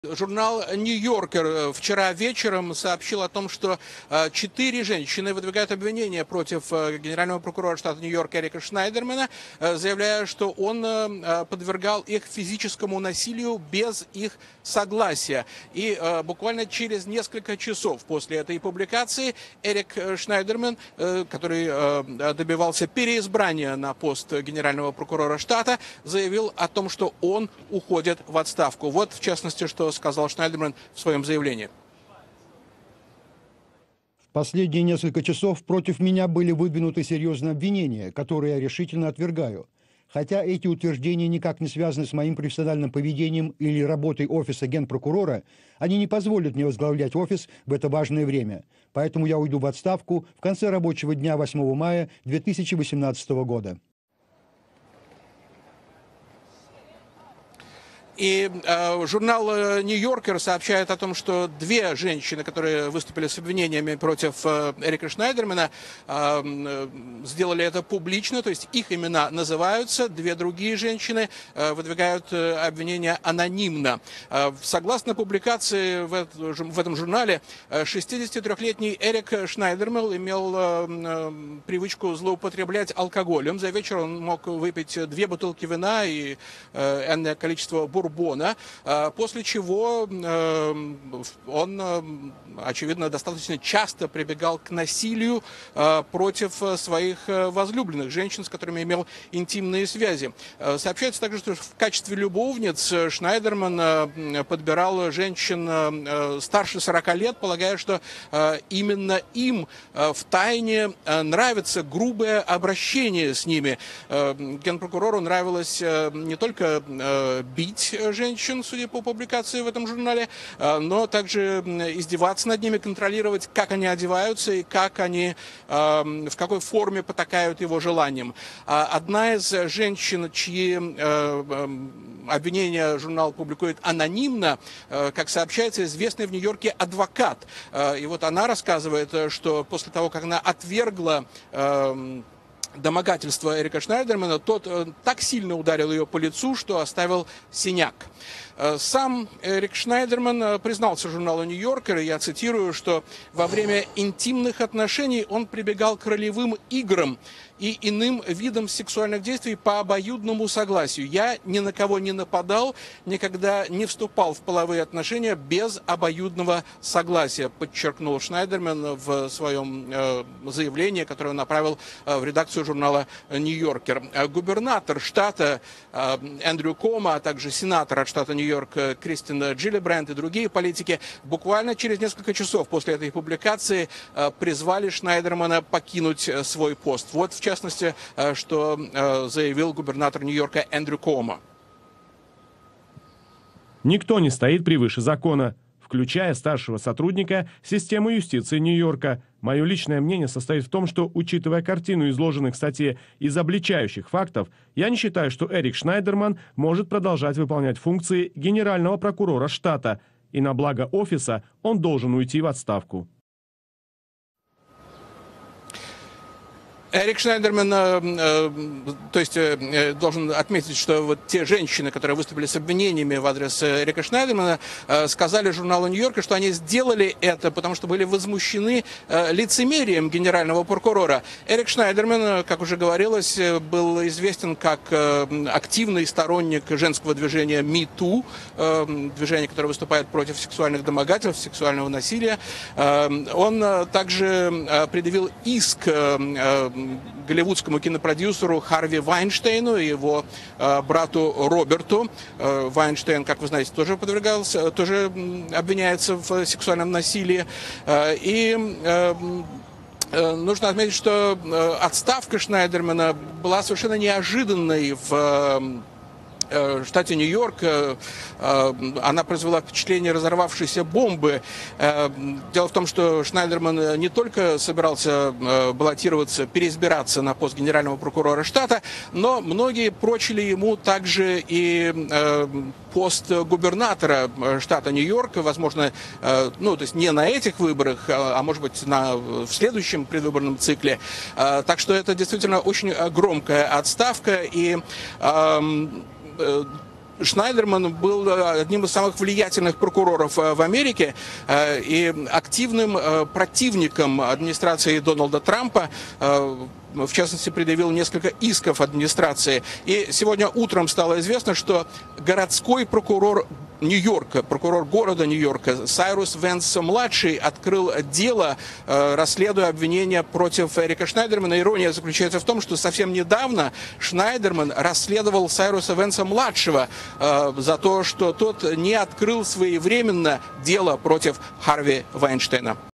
Генпрокурор штата Нью-Йорк ушел при пикантных обстоятельствах
Эрика Шнайдермана обвинили в насилии против женщин, с которыми он имел интимные связи